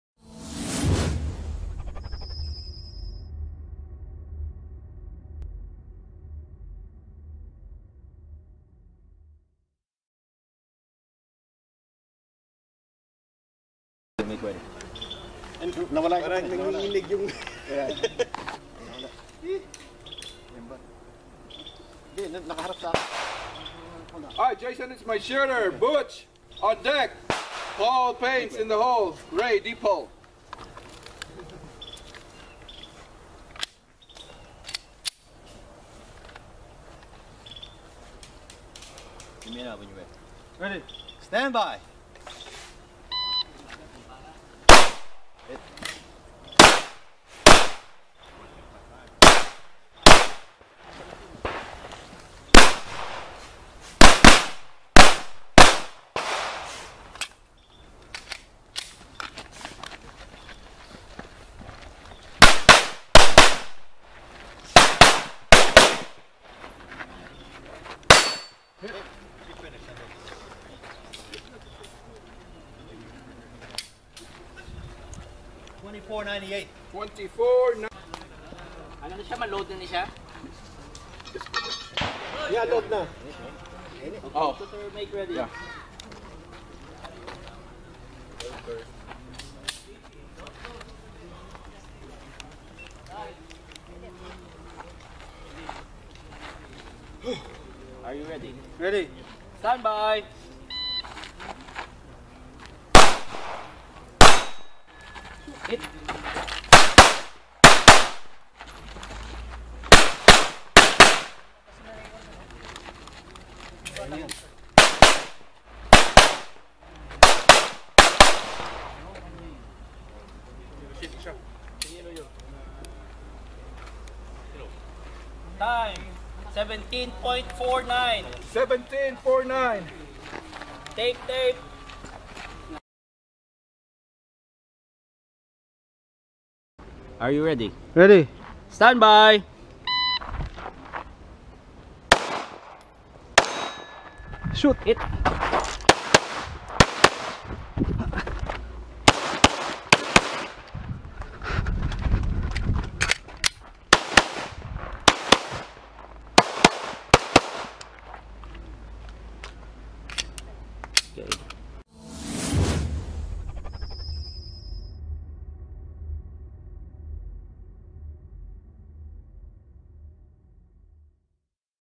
HPGC ANNIVERSARY SHOOT 2016
Houston Pinoy Gun Clun IDPA 6th anniversary fund raising shoot . Part of the proceeds of this shoot will go to Ima's Home for children.